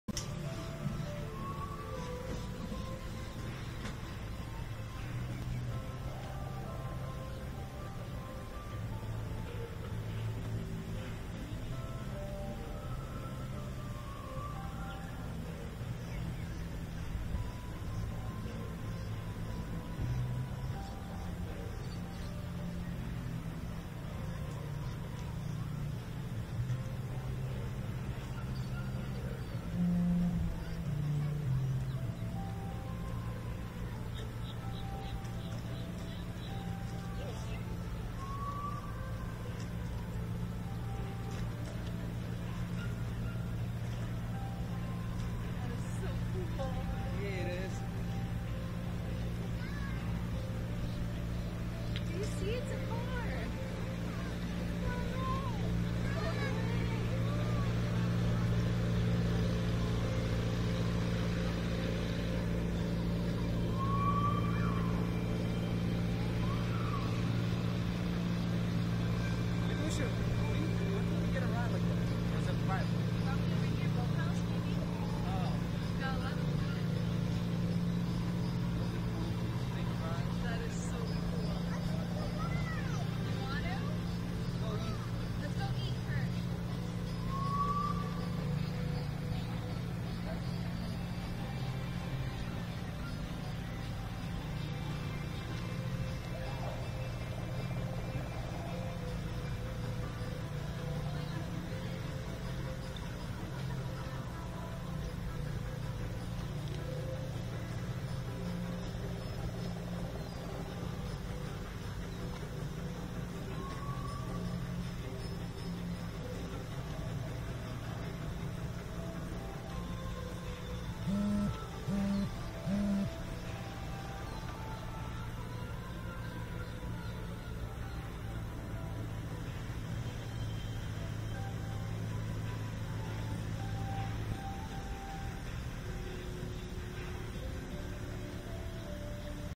Amphicar - Dream Boat - Disney Springs Transforming Car Experience
Amphicar - Dream Boat - Disney Springs Transforming Car Experience wearing daVideo Camera Sunglasses

The Amphicar Model 770 is an amphibious automobile which was launched at the 1961 New York Auto Show, manufactured in West Germany and marketed from 1961 to 1968. Production stopped in 1965.

Designed by Hans Trippel, the amphibious vehicle was manufactured by the Quandt Group at Lübeck and at Berlin-Borsigwalde, with a total of 3,878 manufactured in a single generation.

A descendant of the Volkswagen Schwimmwagen, the Amphicar offered only modest performance compared to most contemporary boats or cars, featured navigation lights and flag as mandated by the US Coast Guard — and after operation in water, required greasing at 13 points, one of which required removal of the rear seat.

The Amphicar name is a portmanteau of "amphibious" and "car".